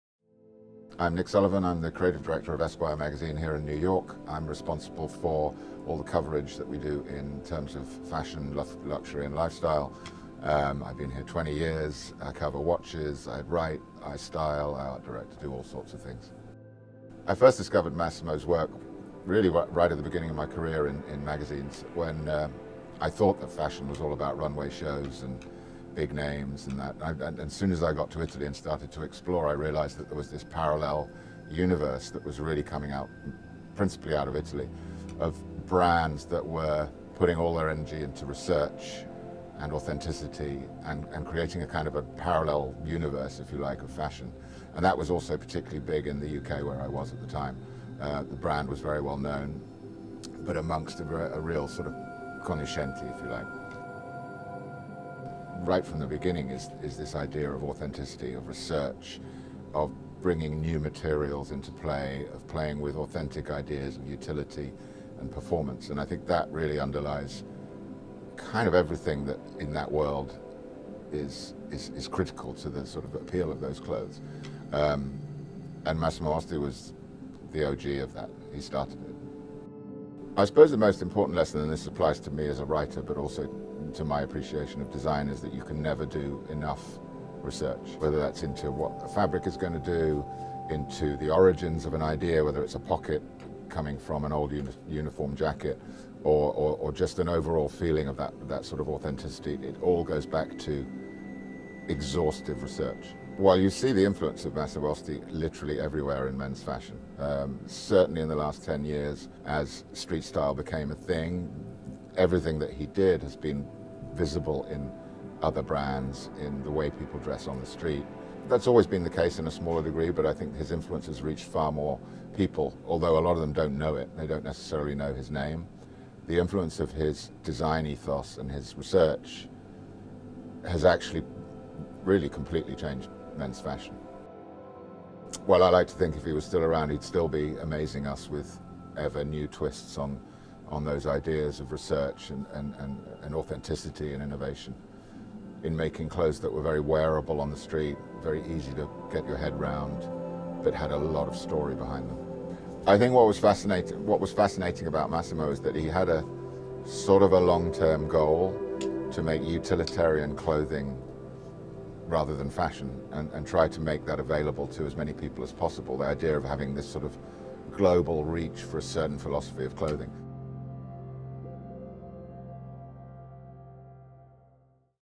A Talk About Massimo Osti With Nick Sullivan
'I thought the world of fashion was just the fashion shows, the big names, then in the magazines I discovered Massimo and the parallel world of utilitarian clothing that was developing in Italy'.

Nick Sullivan, creative director of Esquire magazine, interviewed in his studio in New York recalls Massimo Osti's work as a moment of radical change in modern menswear, thanks to maniacal research into fabrics, authenticity in designs and the creation of 'uniforms studied in every detail with a long history behind them'.